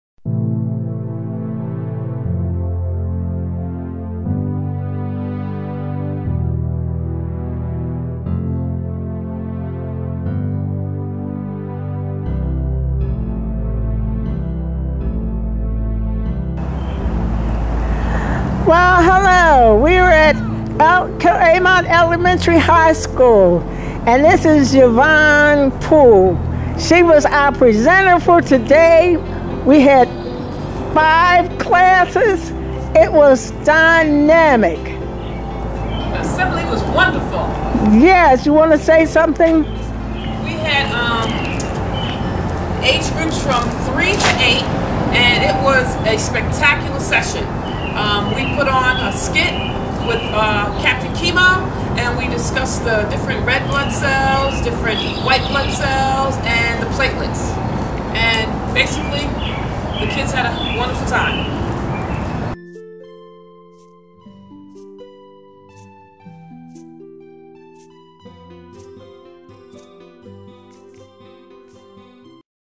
Claymont Elementary School Assembly
School & Youth fundraiser assembly. At least five classes attended the assembly at different times. Children today like volunteering in skits and ask many questions.